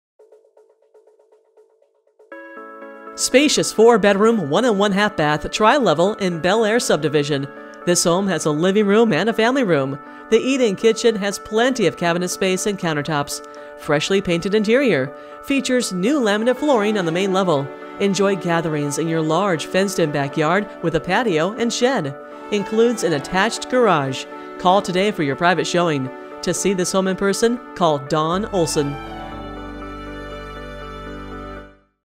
37 Bonds Drive Bourbonnais, IL | ColdwellBankerHomes.com
37 Bonds Drive, Bourbonnais, IL

Spacious four bedroom, one and one-half bathroom tri-level in Belle Aire Subdivision! This home has a iving Room and Family Room. The eat-in kitchen has plenty of cabinet space and counter tops! Freshly painted interior. Features new laminate flooring on main level. Enjoy gatherings in your large fe